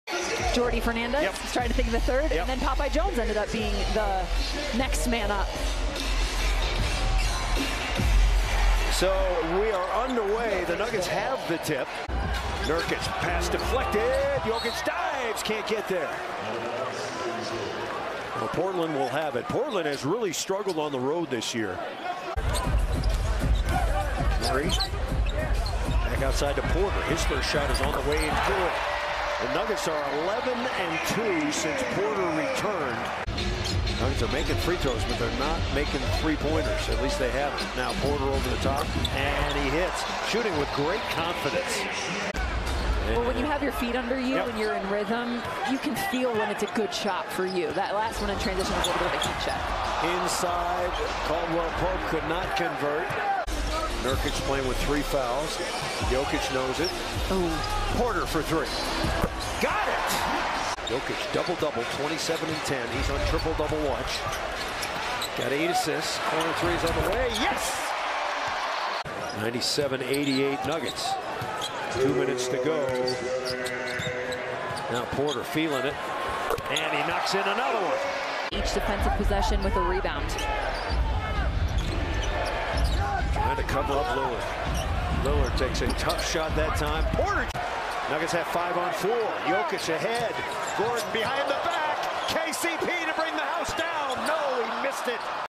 Michael Porter Jr. vs Portland Trail Blazers | Jan 17, 2023
23 PTS
4 REB
0 AST

Michael Porter Jr. vs Portland Trail Blazers
Portland Trail Blazers vs Denver Nuggets